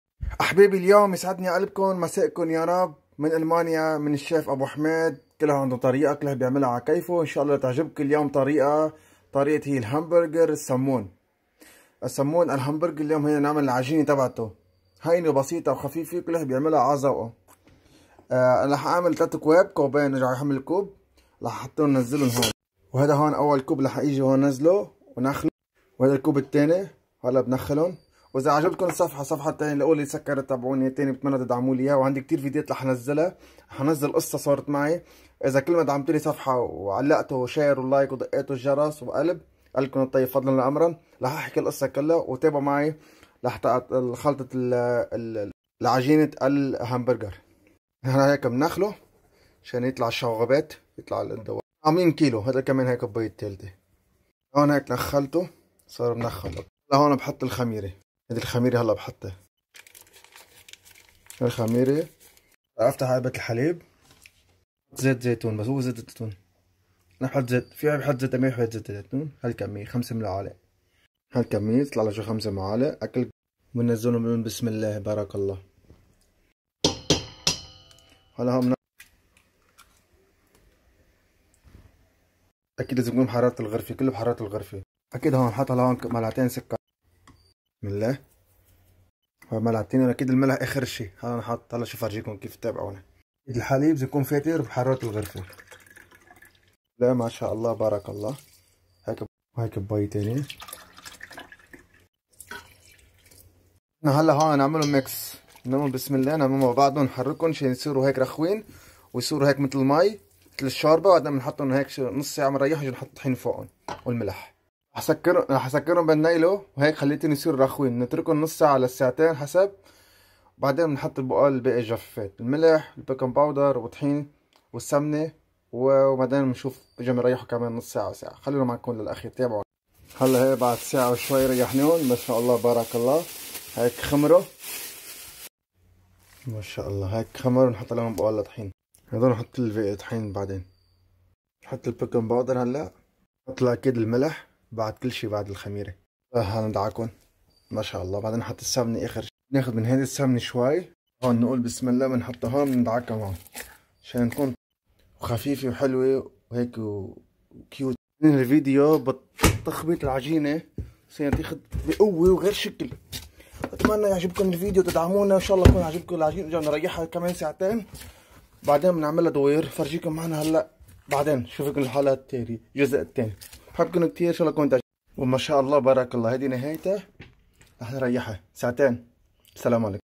حبه السكر اللي بتذوب بتم العجينه الطيبه الخلطه السريه خلطه العجينه الهمبرجر الطريه والطيبه اتمنى
حبه السكر اللي بتذوب بتم العجينه الطيبه الخلطه السريه خلطه العجينه الهمبرجر الطريه والطيبه اتمنى تعجبكم مثل الاسفنجه مثل القلب الطري مثل قلوبكم الطيبه يوصل الفيديو اكسبلور من فضل الله وفضل الله وشكرا @الهيام🚶‍♂️❤️ @ABUPETER